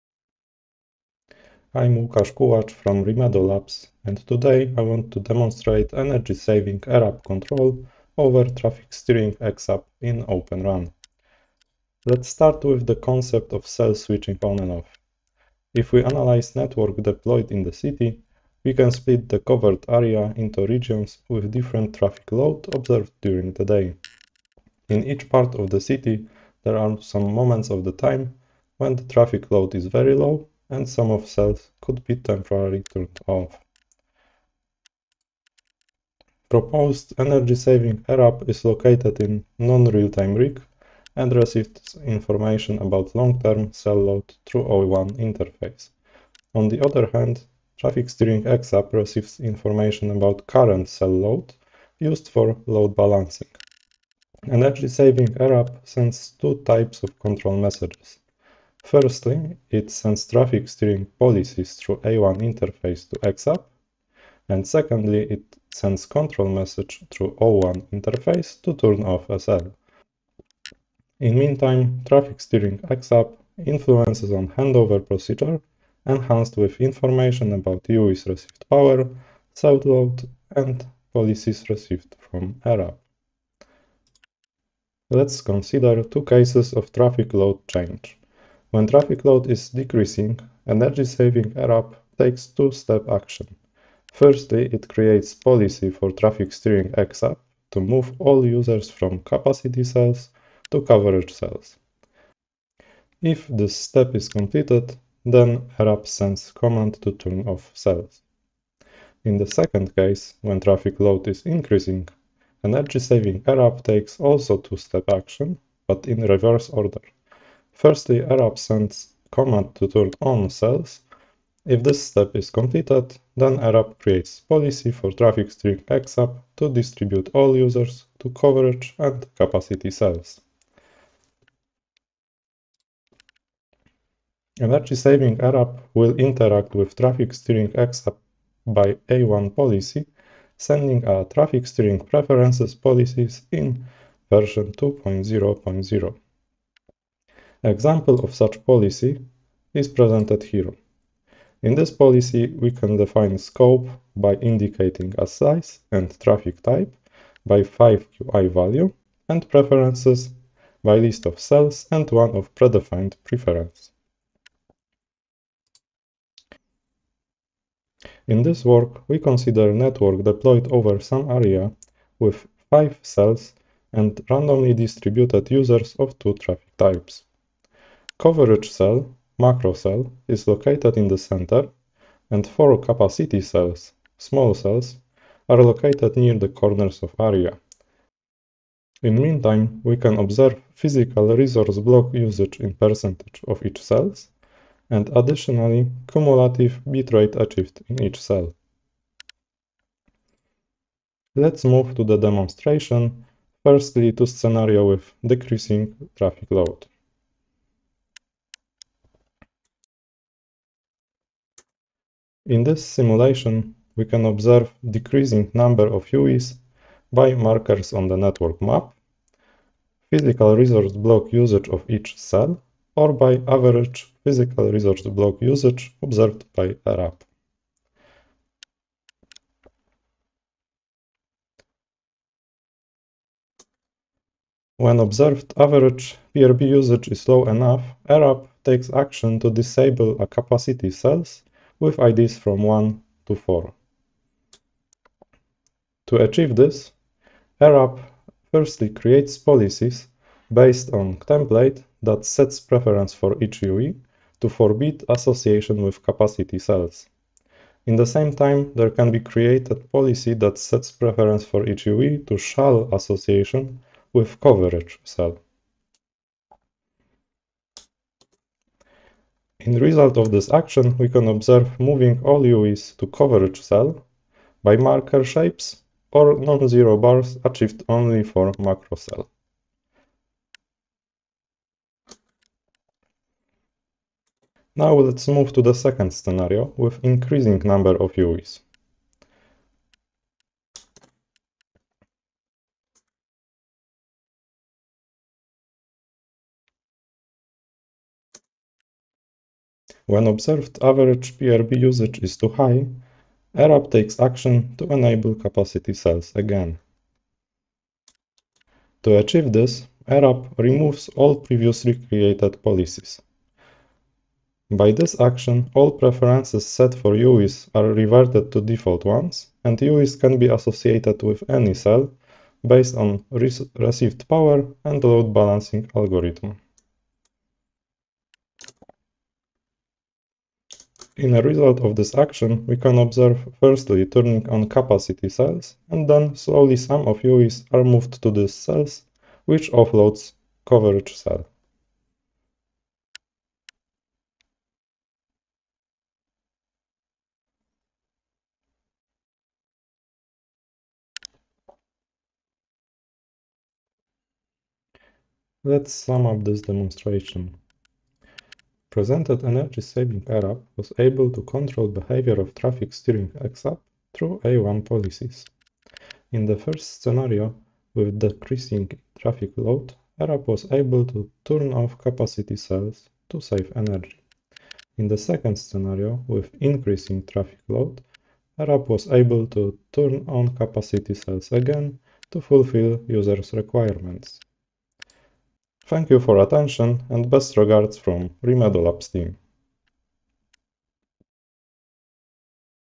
Energy Saving rApp control over Traffic Steering xApp for Open RAN (Demo)
In this video, we present an innovative approach to energy saving using the RIC, which is based on the direct cooperation of Traffic Steering xApp (TS-xApp) and Energy Saving rApp (ES-rApp), both developed by Rimedo Labs.

The TS-xApp can simultaneously distribute the load between network nodes and offload specific traffic to dedicated cells. The goals (or objectives) can be defined by the operators and the xApp can be controlled with the use of policy delivered from the A1 interface.

ES-rApp controls the operation of TS-xApp by creating appropriate traffic control policies. The simplest energy-saving situation can be illustrated as a situation when there is a very small load in the network and ES-rApp decides to redirect traffic from one of the cells to the rest of the network. Then, it is possible to turn off (or enable sleep mode) a temporarily redundant cell, which in turn reduces the energy consumed in the network.

In this demo, we show cell on/off use cases in a HetNet scenario with macro and small cells. The demonstration shows the operation of both applications working simultaneously, yet independently in two cases - decreasing traffic load and increasing traffic load. The setup uses the VMware distributed RIC.

To get a more high level overview of the use case and operations, check out this video